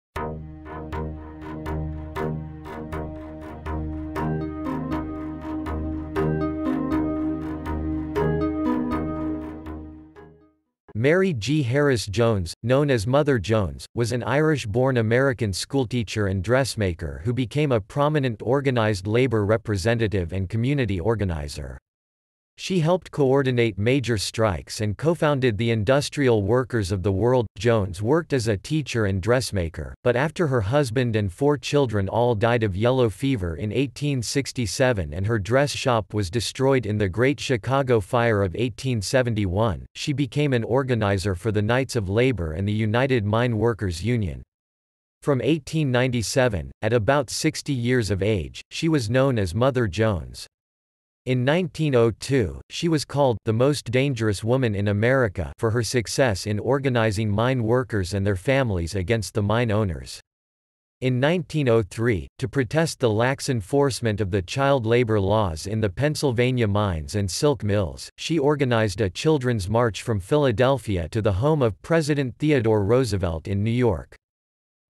What is Mary Harris Jones?, Explain Mary Harris Jones, Define Mary Harris Jones
~~~ Mary Harris Jones ~~~

Title: What is Mary Harris Jones?, Explain Mary Harris Jones, Define Mary Harris Jones

Description: Mary G. Harris Jones , known as Mother Jones, was an Irish-born American schoolteacher and dressmaker who became a prominent organized labor representative and community organizer. She helped coordinate major strikes and cofounded the Industrial Workers of the World. Jones worked as a teacher and dressmaker, but after her husband and four children all died of yellow fever in 1867 and her dress shop was destroyed in the Great Chicago Fire of 1871, she became an organizer for the Knights of Labor and the United Mine Workers union. From 1897, at about 60 years of age, she was known as Mother Jones. In 1902, she was called "the most dangerous woman in America" for her success in organizing mine workers and their families against the mine owners. In 1903, to protest the lax enforcement of the child labor laws in the Pennsylvania mines and silk mills, she organized a children's march from Philadelphia to the home of President Theodore Roosevelt in New York.